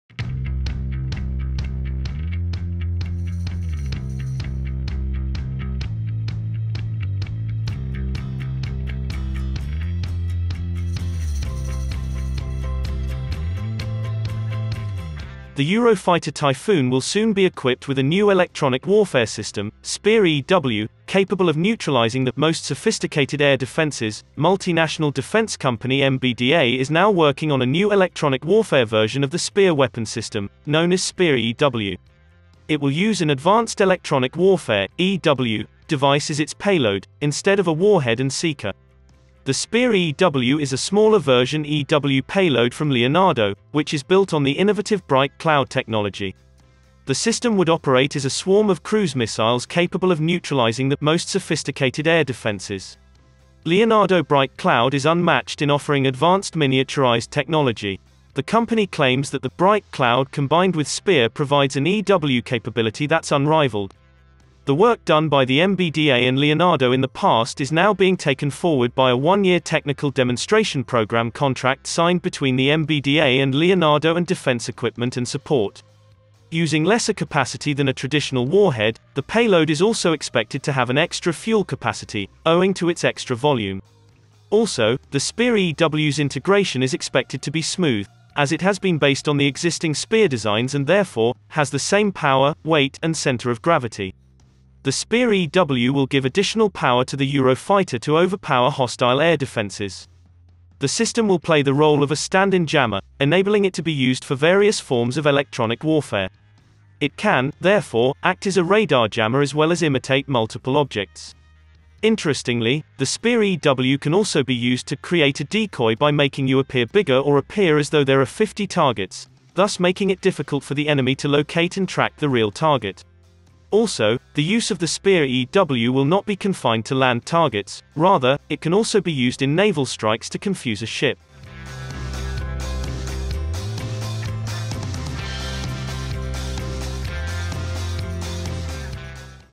The UK Eurofighter Typhoon Will Equipped SPEAR Electronic Warfare Missile
Multinational defense company MBDA is now working on a new electronic warfare version of the SPEAR weapon system, known as SPEAR-EW. It will use an advanced Electronic Warfare (EW) device as its payload, instead of a warhead and seeker.

Interestingly, the SPEAR-EW can also be used to “create a decoy by making you appear bigger or appear as though there are 50 targets”, thus making it difficult for the enemy to locate and track the real target.